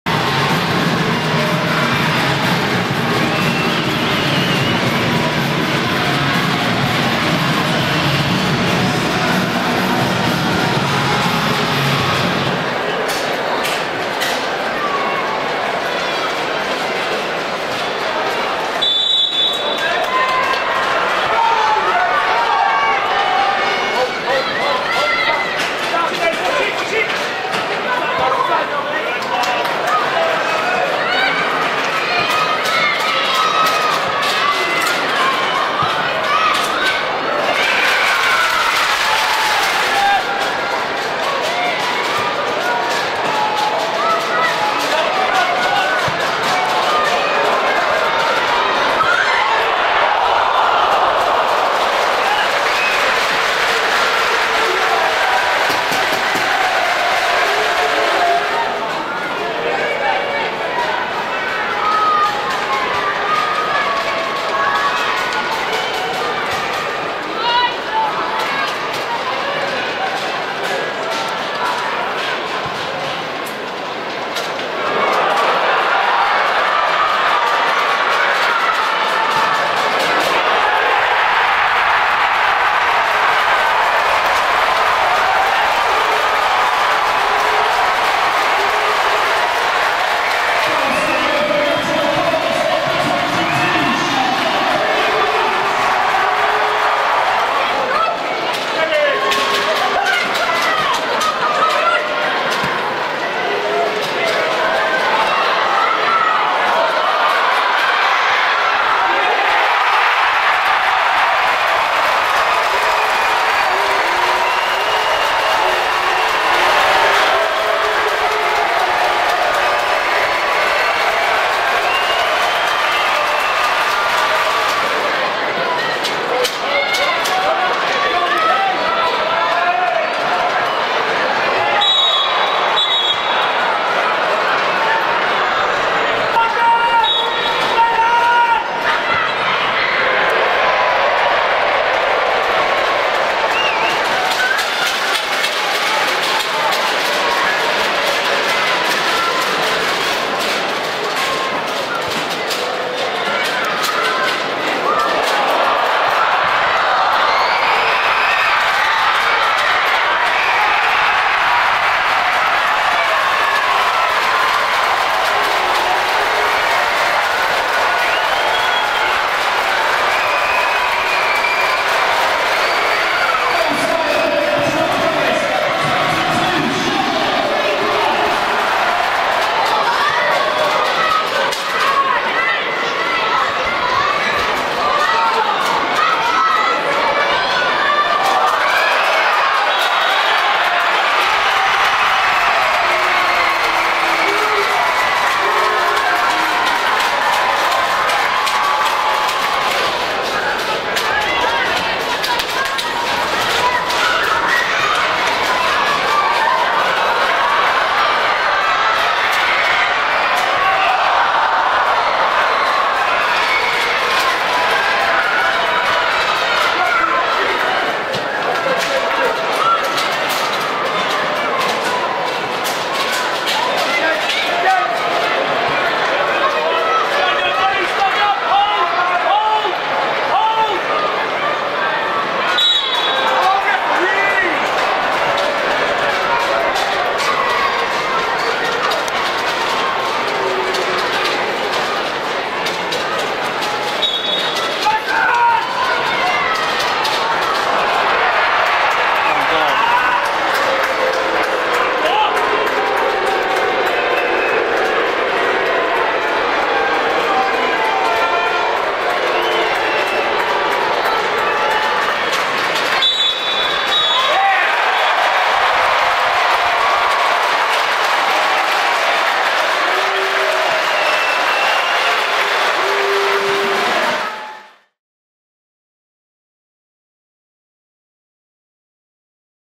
HIGHLIGHTS I Nottingham Forest Women Vs Derby County Women
Highlights from the East Midlands Derby Between Nottingham Forest Women Vs Derby County Women.